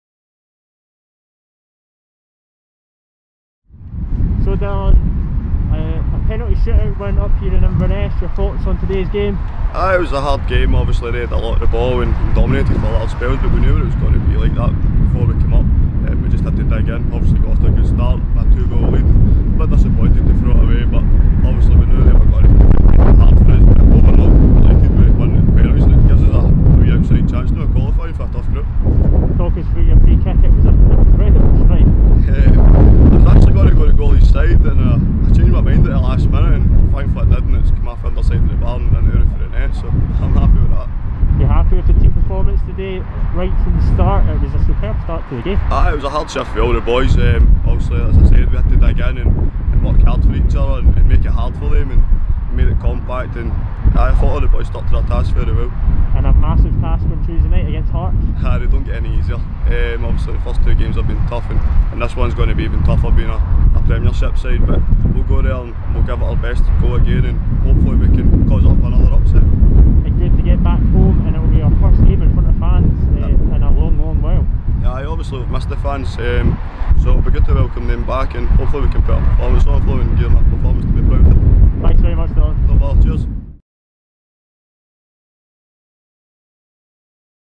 Dylan Mackin Post-Match Interview Vs Inverness Caledonian Thistle 17/07/21
Dylan Mackin speaks to SATV HD following Stirling's 2-2 draw at Inverness in which the Binos then won the extra point in a penalty shootout in the Premier Sports Cup Group A.